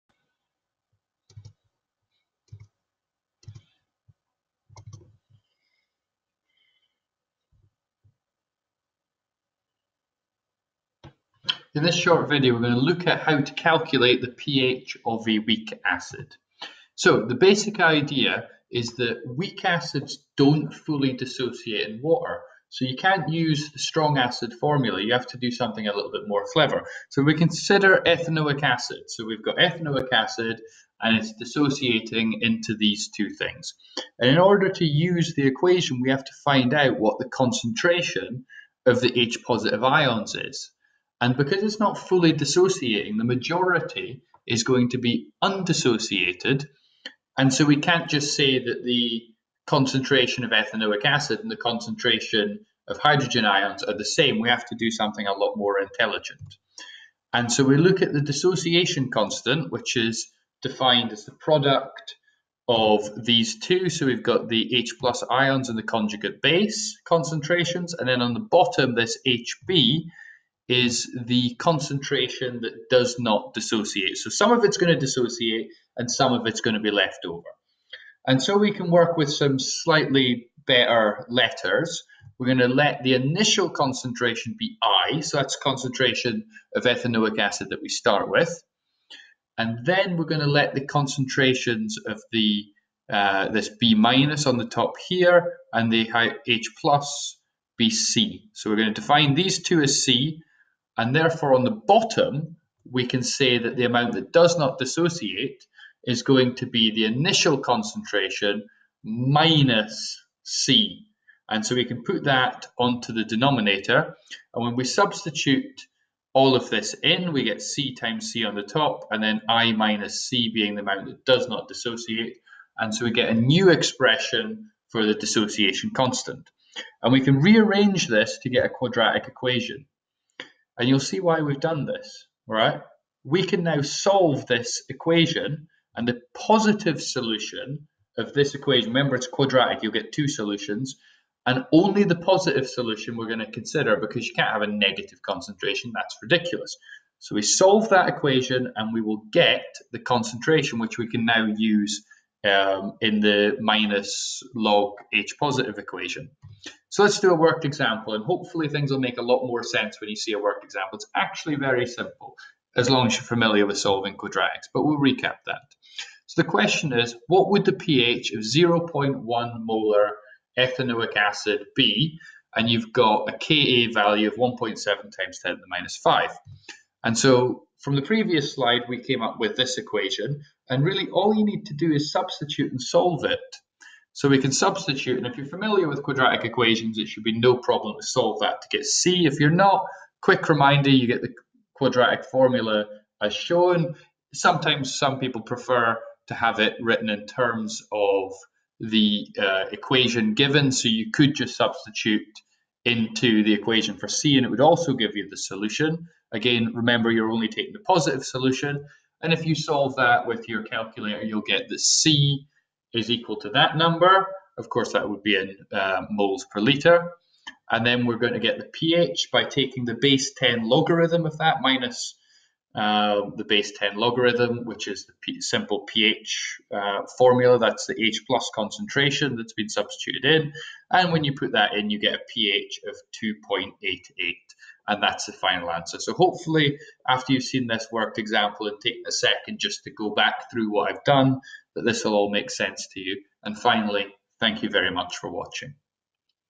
How to Calculate the pH of a Weak Acid
In this video I will teach you how you can calculate the pH of a weak acid using the quadratic formula to find the concentration of the dissociated hydrogen ions.